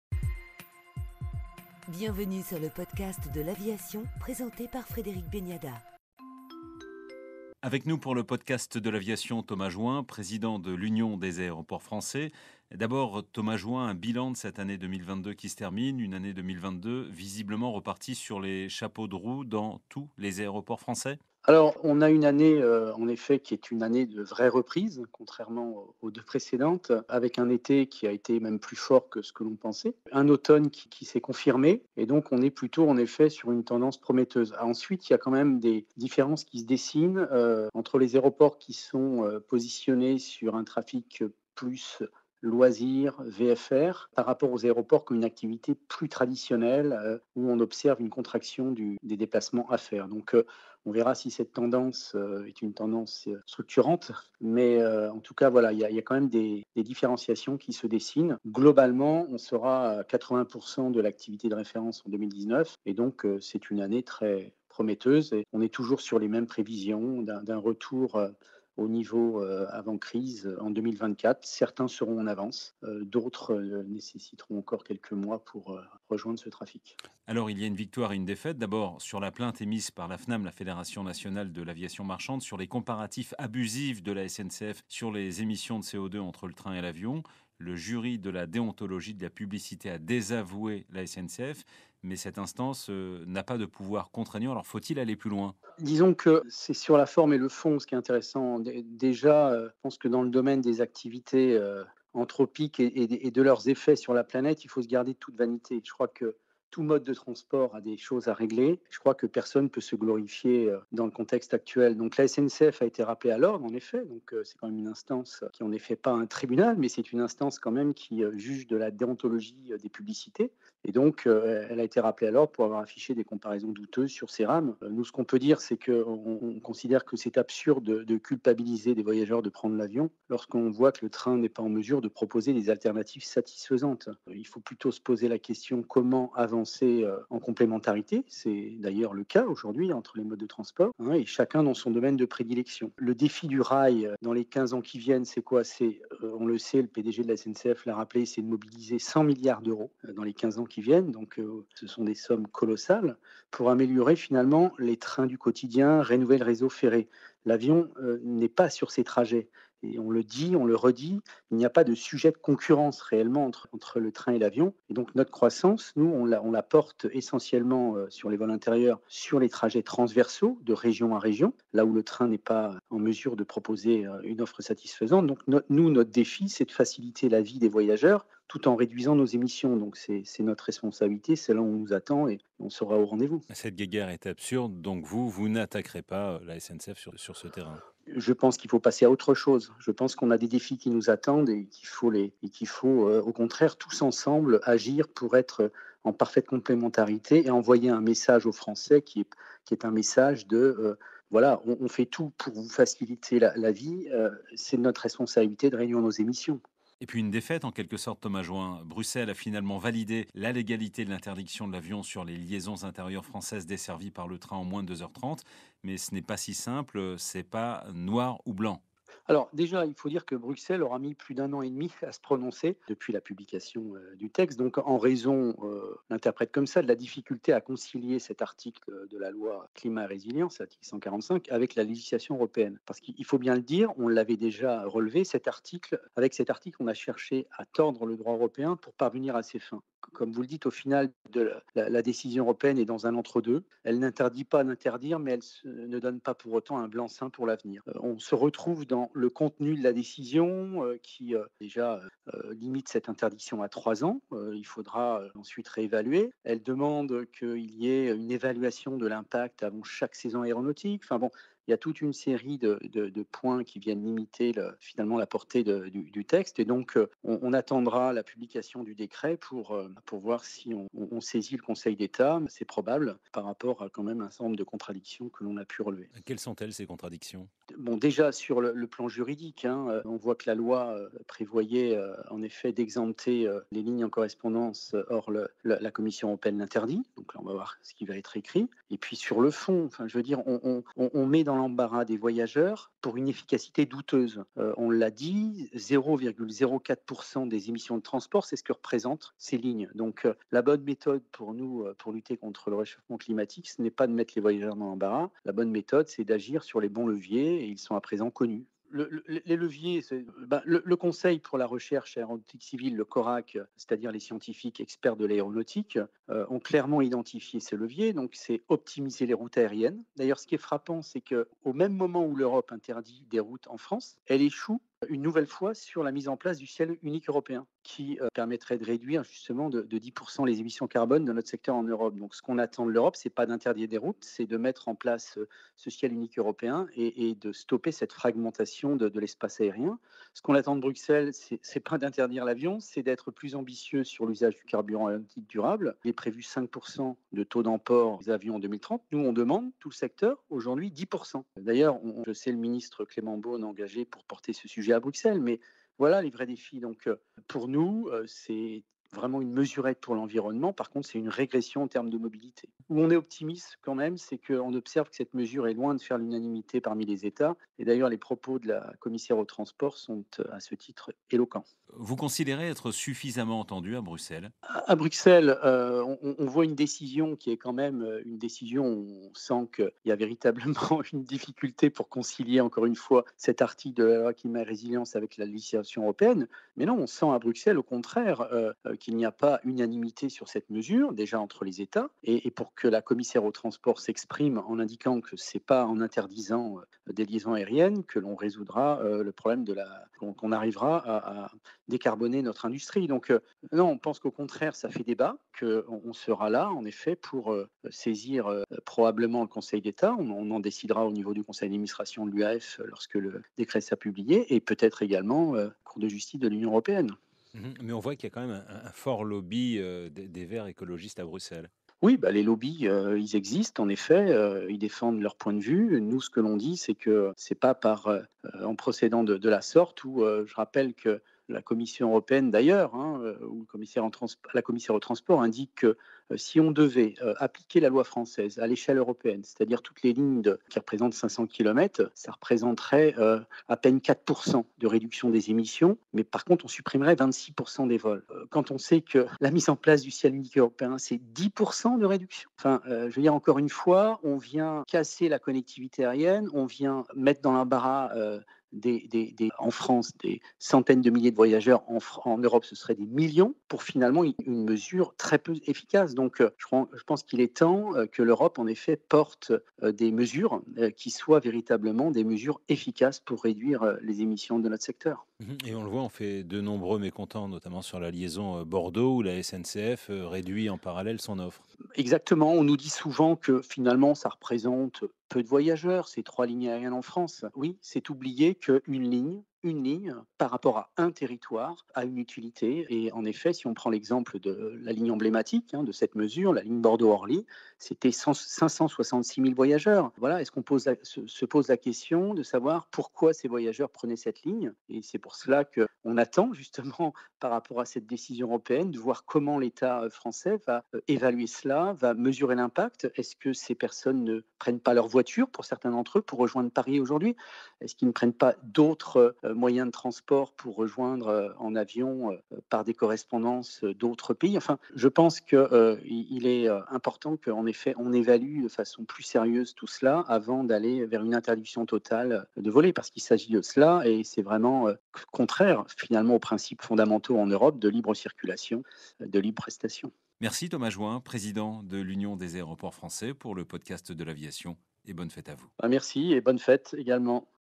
Entretien avec Thomas Juin, président de l'UAF & FA
« Au même moment où l'Europe interdit des routes en France, elle échoue une nouvelle fois sur le ciel unique européen »

Thomas Juin revient brièvement sur l'été meilleur que prévu et l'automne très positif que les aéroports ont vécu, tout en rappelant les disparités dans cette reprise. Satisfait du jugement du jury de déontologie publicitaire, qui a estimé que la publicité de la SNCF comparant les émissions du train et de l'avion était mensongère, il répète que les deux moyens de transport ne sont pas concurrents mais totalement complémentaires.

Il se montre en revanche plus partagé sur la décision de l'Union européenne de valider le projet de loi français sur l'interdiction des lignes aériennes lorsque le train propose une alternative en moins de 2h30. Alors que l'UE a mis des conditions qui imposent de réellement évaluer l'impact écologique de la mesure, Thomas Juin souligne quelle est totalement inefficace pour décarboner l'aviation et que d'autres mesures devraient être portées par lUE : soutien sur les carburants durables, ciel unique européen.